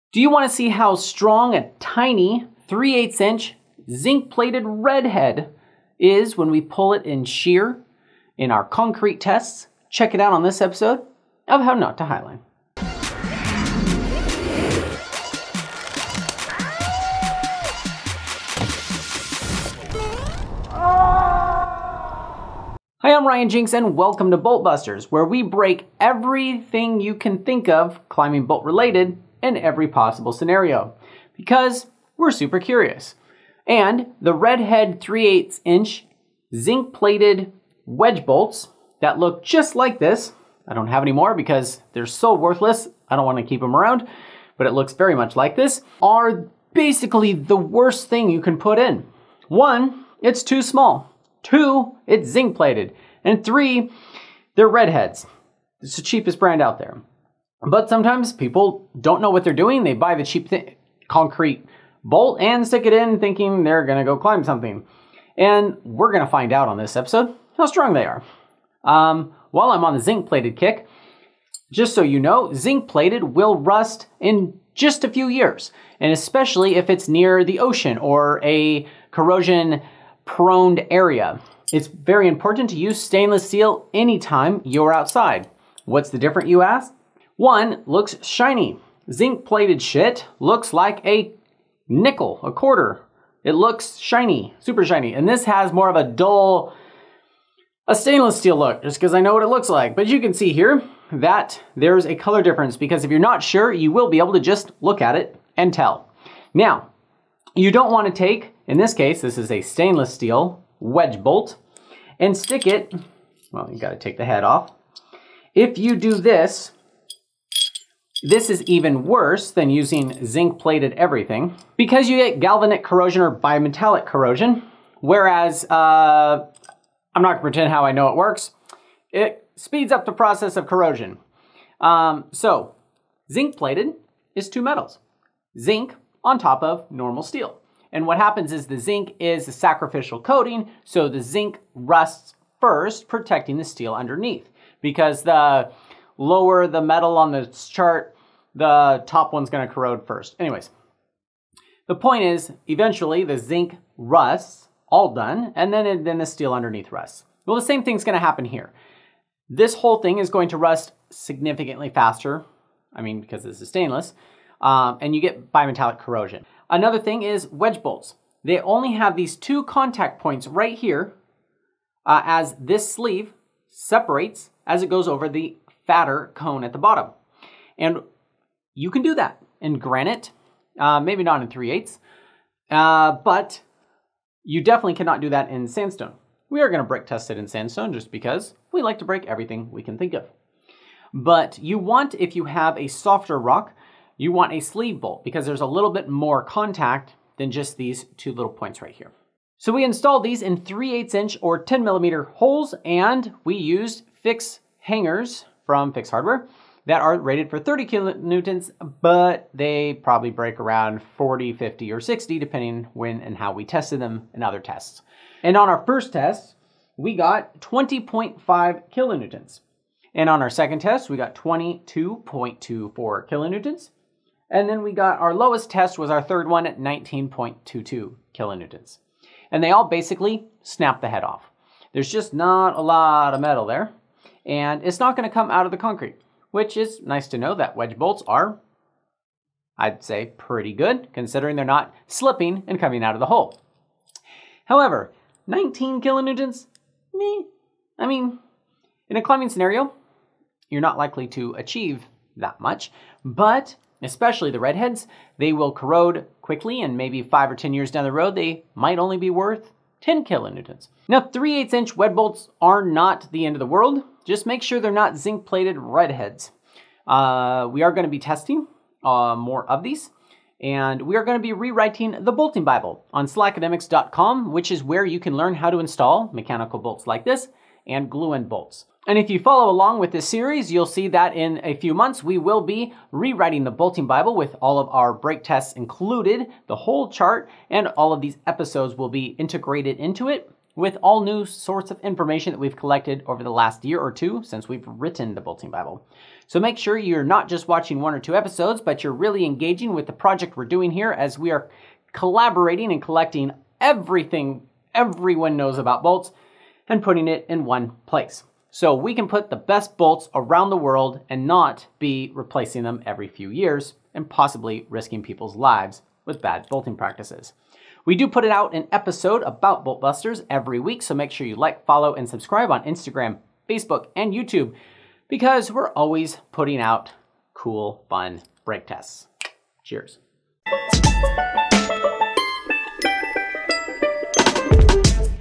Bolt Buster - ⅜” zinc plated red heads - climbing and highline bolt break test
We tested the lowest bolt on the bolting totem pole.  Cheap red heads.  ⅜” red heads broke in shear at 20.50kn and 22.24kn and 19.22kn.  Please don't bolt if you don't need to and use titanium if near the ocean and never use plated steel bolts outside.  With Bobby Hutton